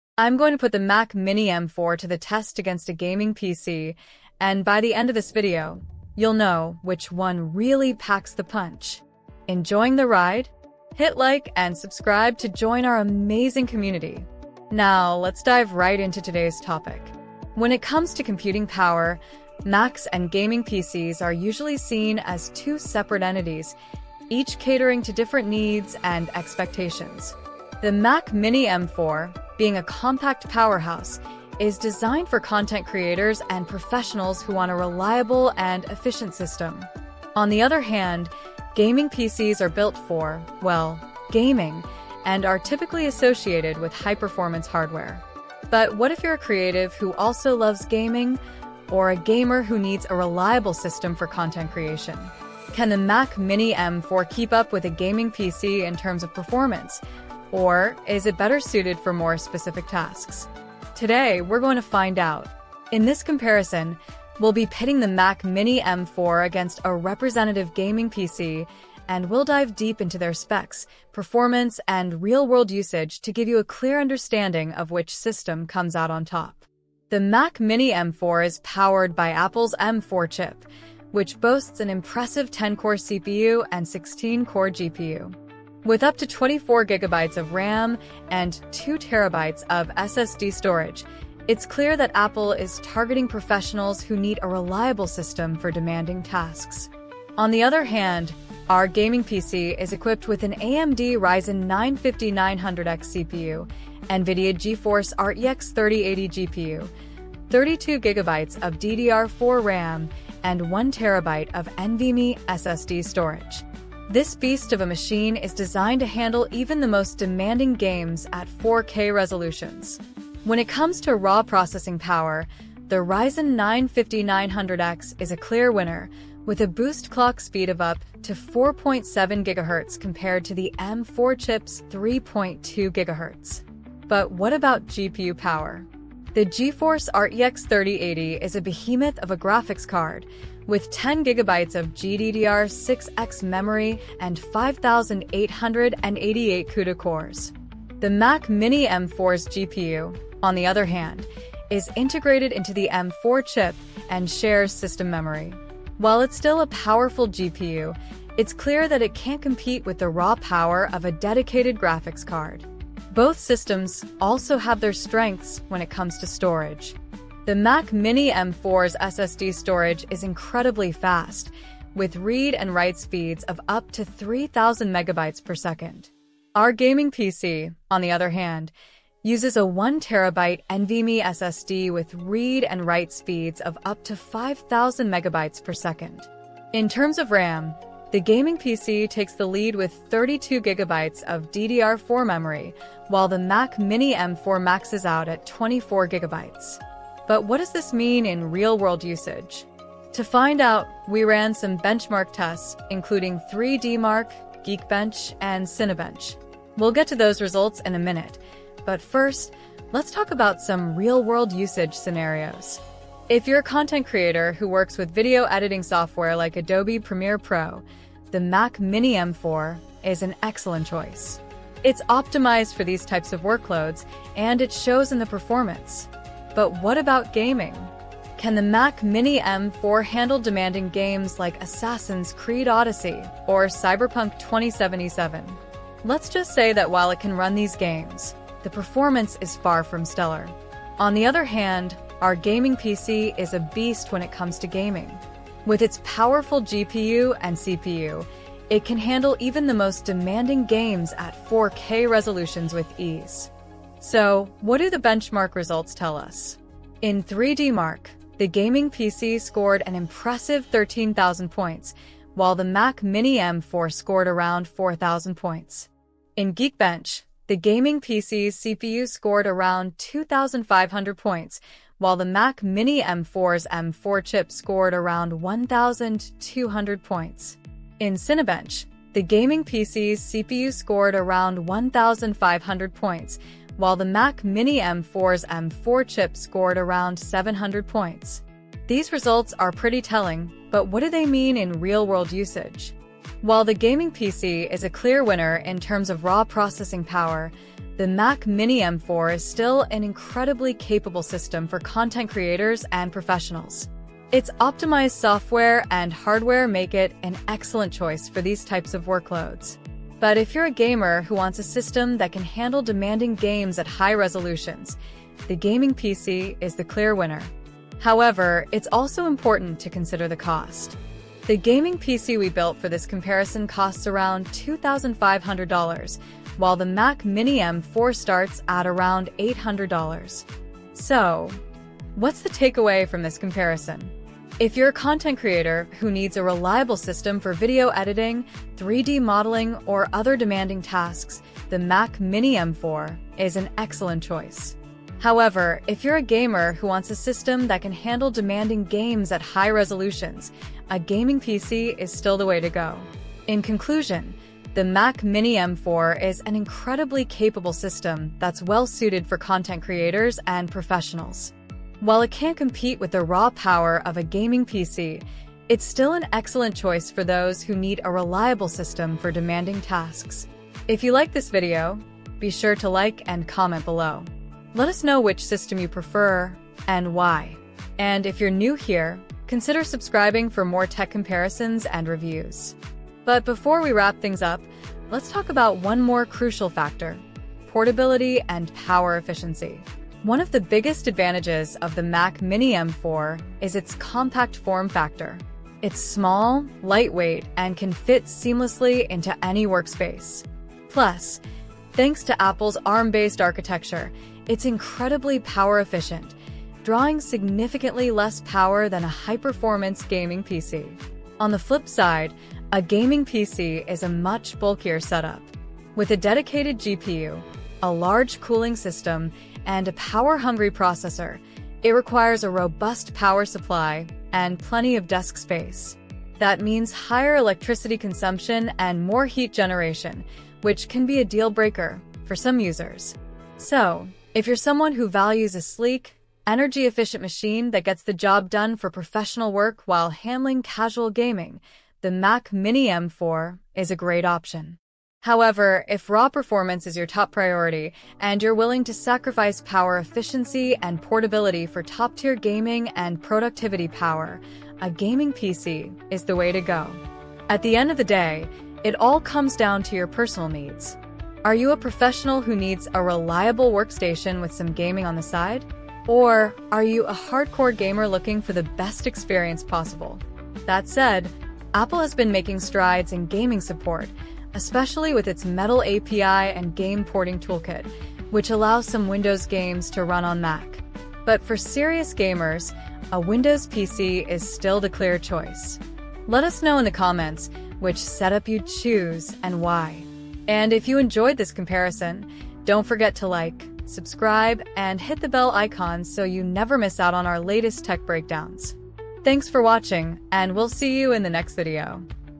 Mac Mini M4 vs Gaming PC: The Ultimate Showdown
In this video, we'll put the Mac Mini M4 to the test to see if it can hold its own against a gaming PC. Can Apple's compact powerhouse keep up with the performance of a custom-built gaming rig? We'll run a series of benchmarks and real-world tests to find out. From gaming performance to content creation and general productivity, we'll see where the Mac Mini M4 shines and where it falls short. If you're considering upgrading to a Mac Mini M4 or building a gaming PC, this video is for you. Watch to the end to find out which one comes out on top!